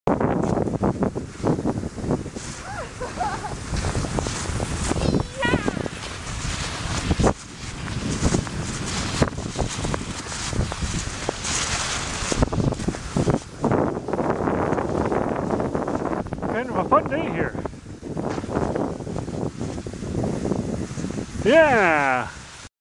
Blue Hills Trail February 28, 2015
Great cross country skiing conditions at northern Wisconsin's Blue Hills Trail on the last day of February 2015